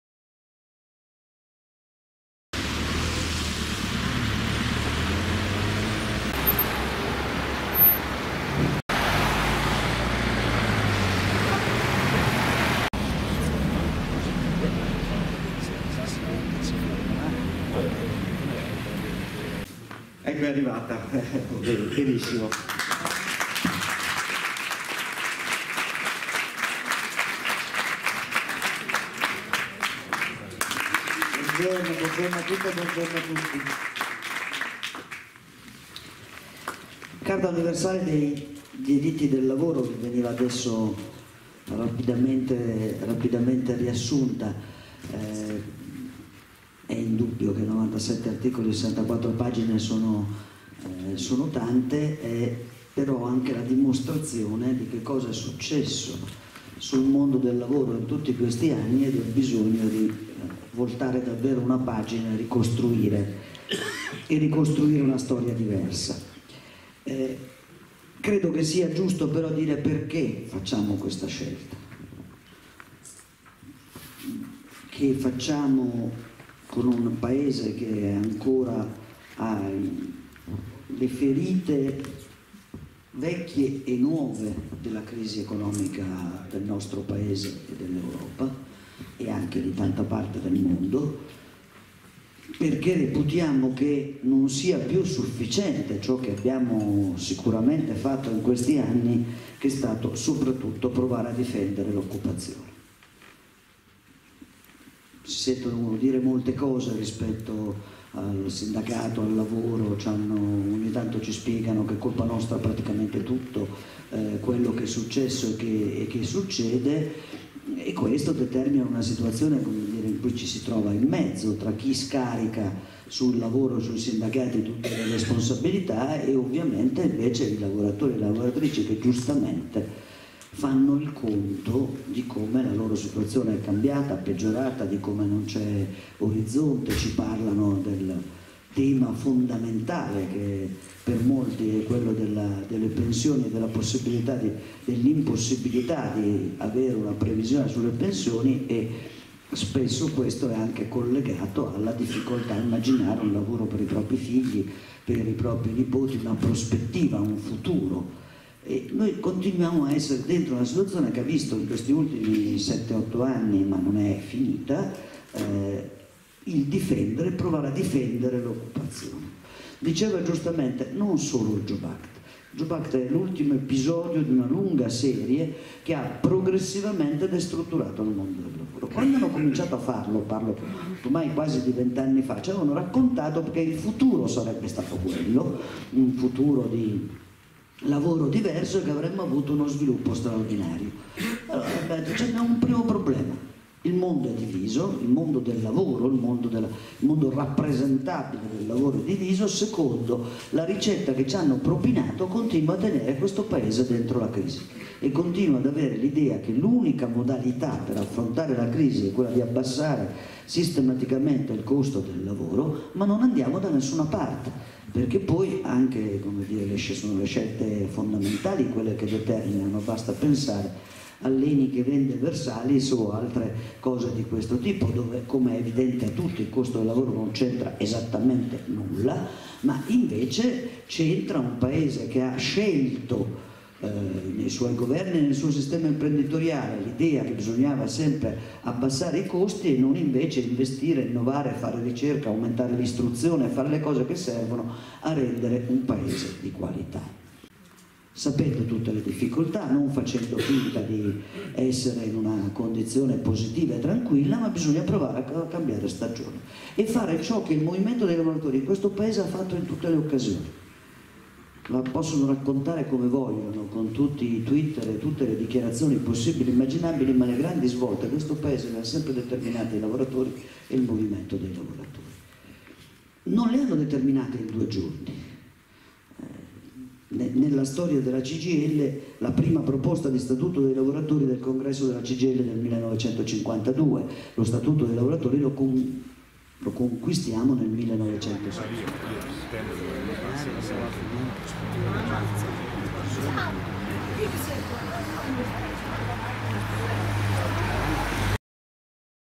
Susanna Camusso in Riviera del Brenta
Fiesso D'Artico-Venezia. Susanna Camusso era in Riviera del Brenta quest'oggi, ha incontrato i lavoratori del settore Tessile, e Calzaturiero; il tema discusso era la "carta dei Diritti Universali del Lavoro". L'ex cinema della parrocchia di Fiesso D'Artico era stracolmo, nonostante la pioggia, in molti hanno lasciato il posto di lavoro per seguire quello che il Segretario generale della CGIL doveva dire. Tante parole, Poche domande, pochi applausi, poche risposte.